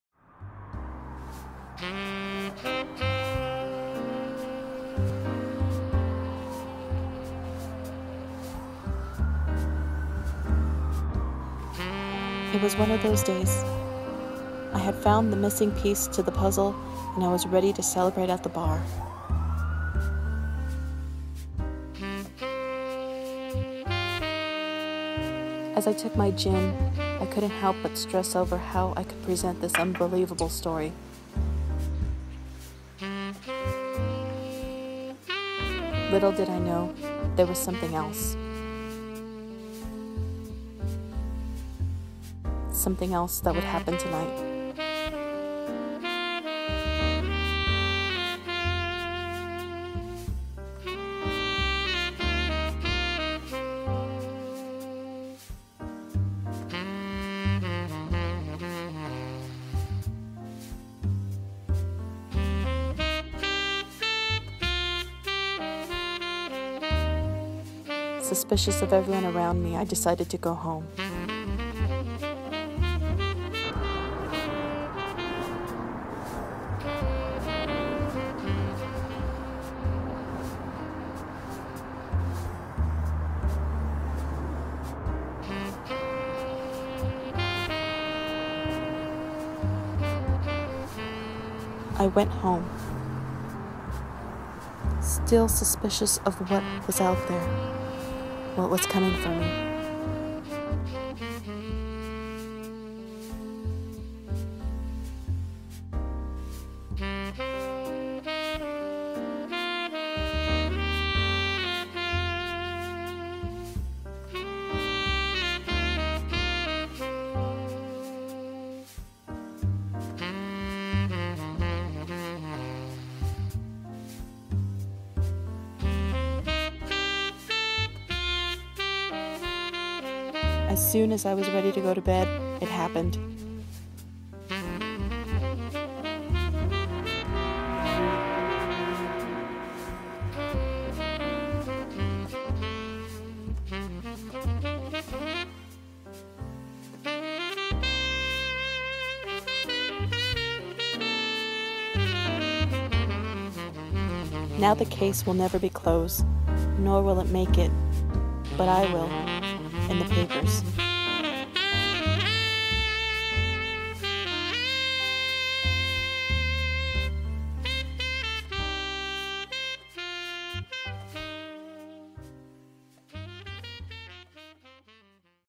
Clues to nowhere
Class project inspired by Film Noir era.
Directed/ filmed and edited by Cassandra Rincon
Writer and Actress: Brianna Hernandez